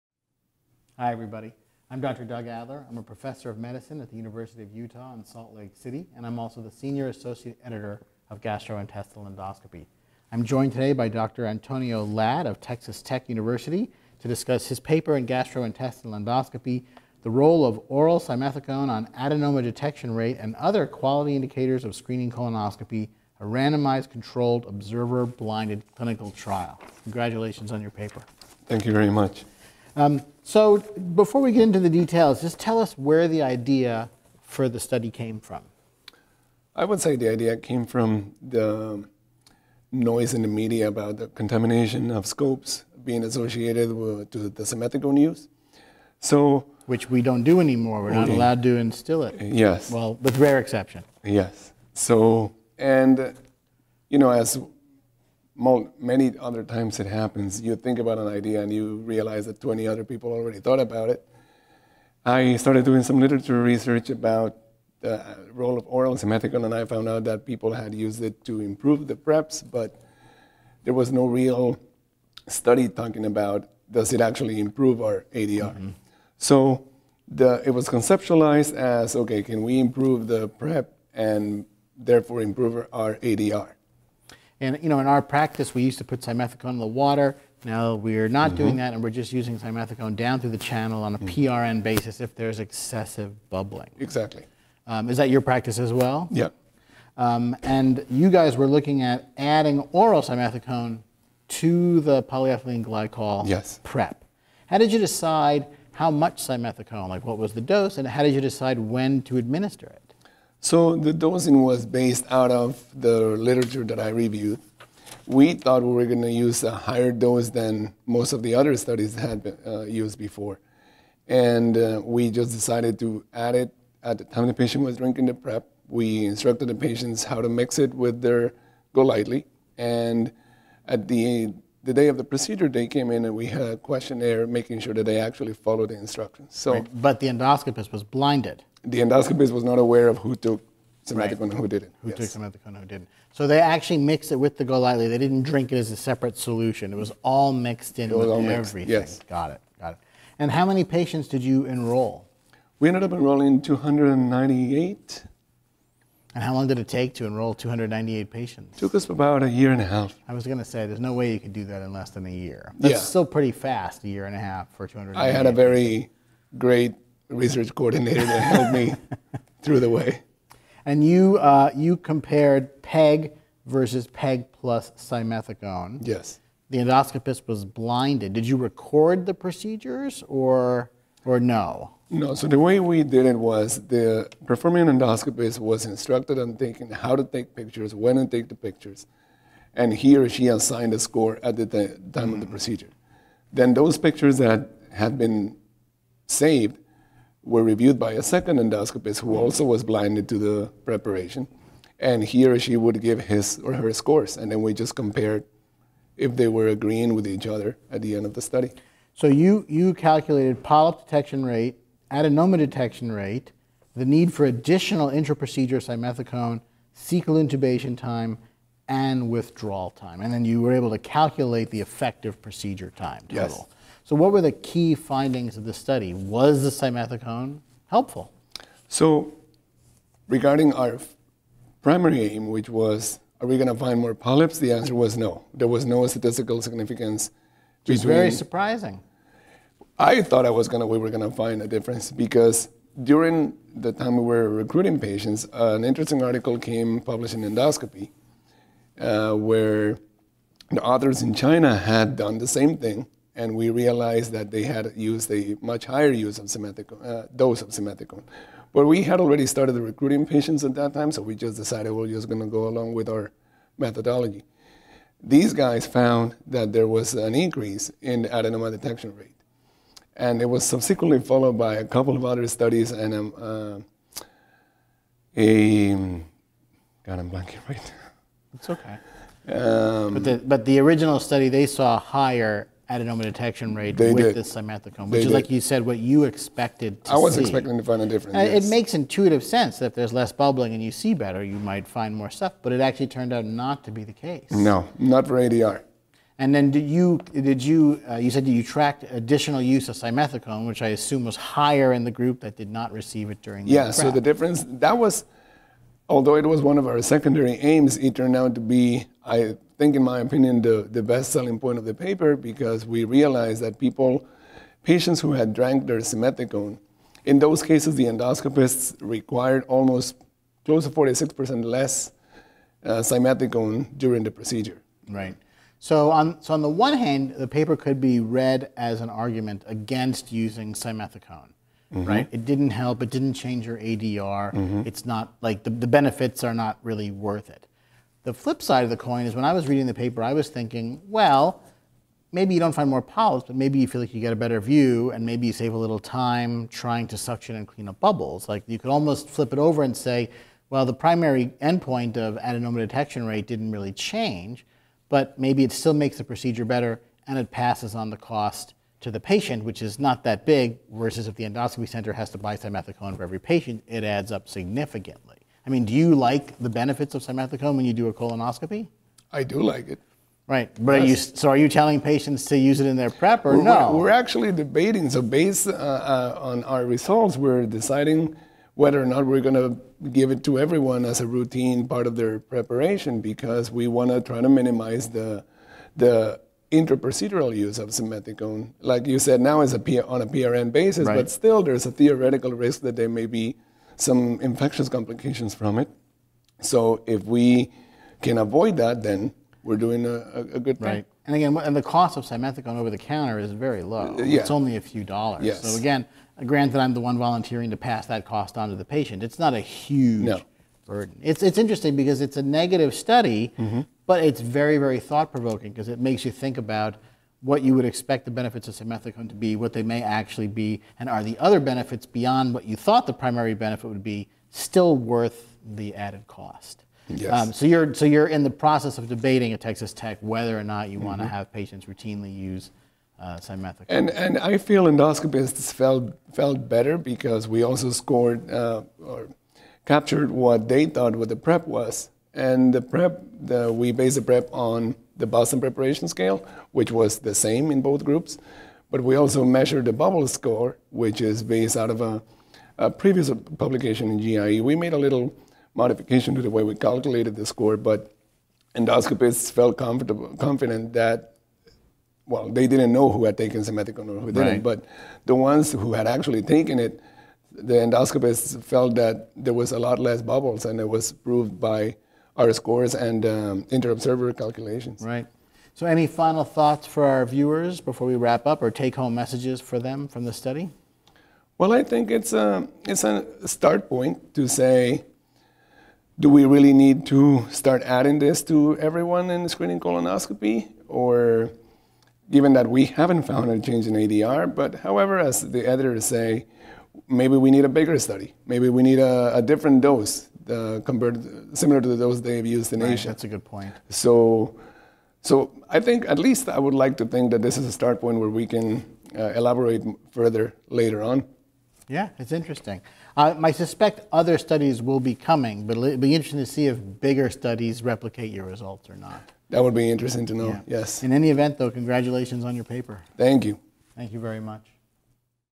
2019 July Author Interview-Antonio Mendoza Ladd
Dr Antonio Mendoza Ladd discusses his article, "The role of oral simethicone on adenoma detection rate and other quality indicators of screening colonoscopy: a randomized, controlled, observer-blinded clinical trial" from the July issue.